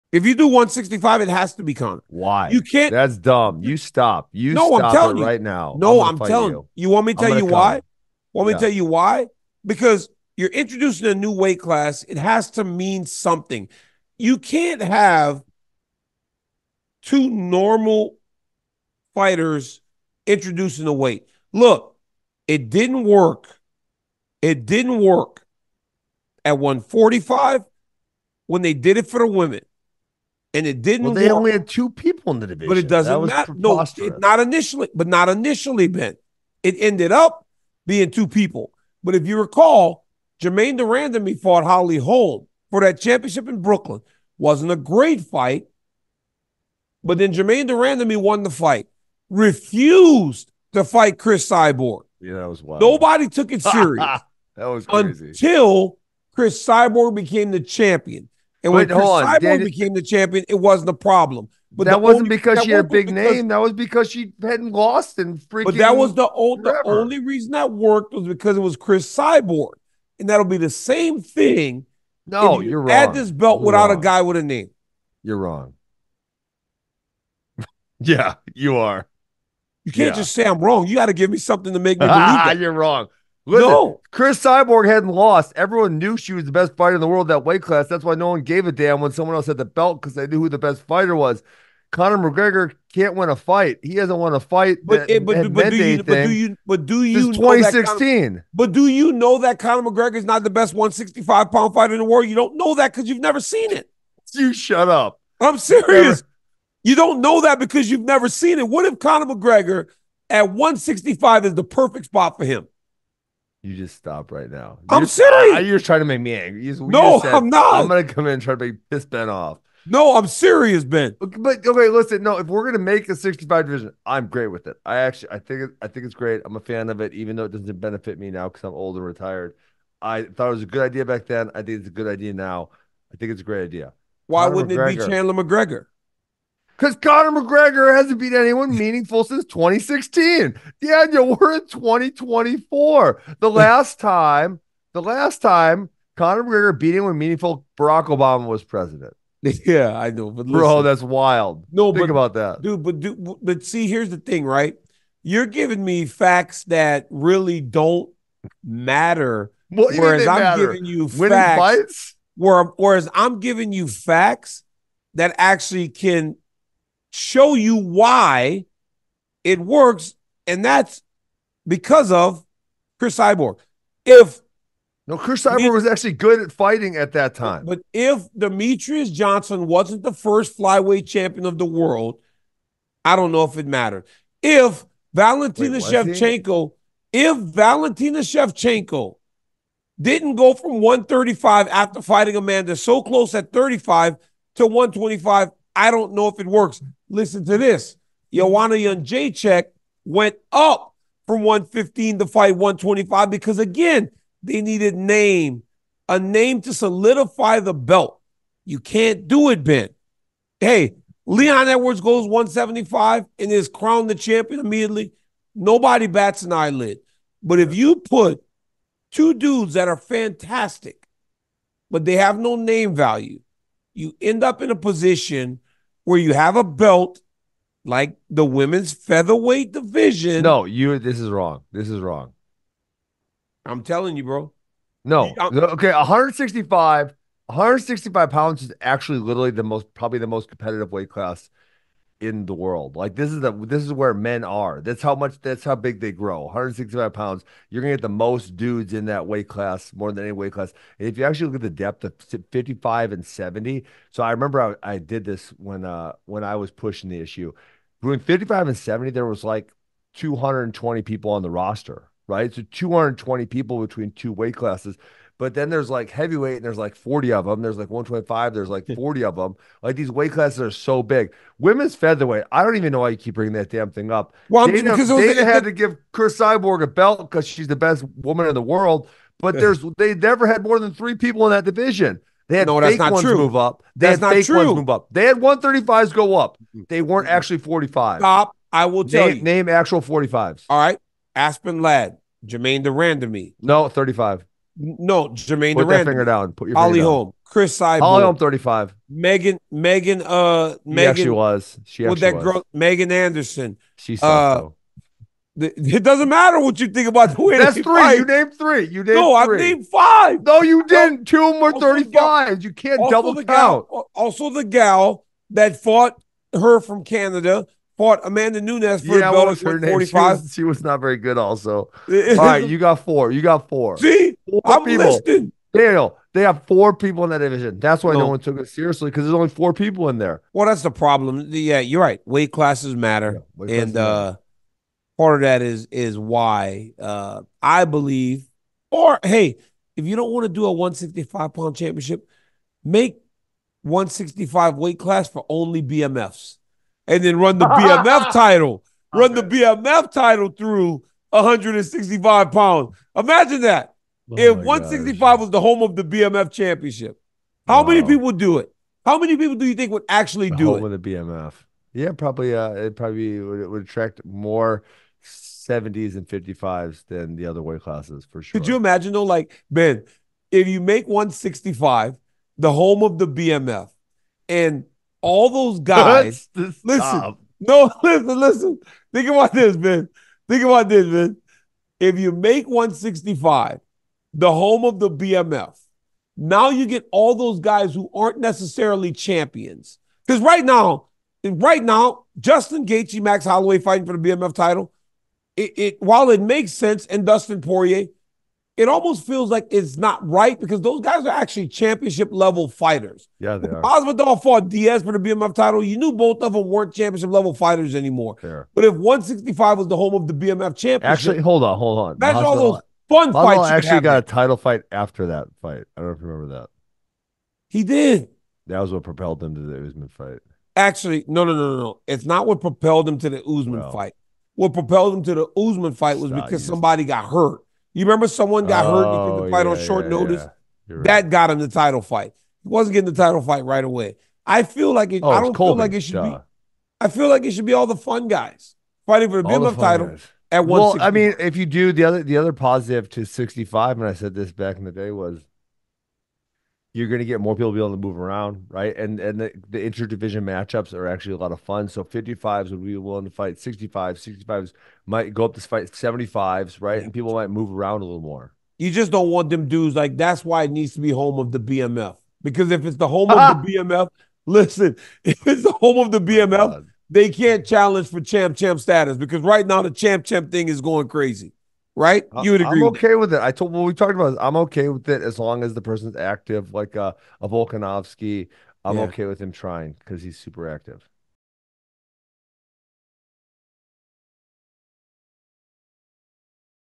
Ben Askren ADAMANT Conor McGregor CANNOT FIGHT for BMF or UFC title yet | Daniel Cormier Highlights
Daniel Cormier and Ben Askren GET INTO IT on whether or not Conor McGregor should make his return in a title fight at 165 pounds! Plus, DC tells Askren a WILD THEORY on the 165-pound division being "the new home" of the BMF title.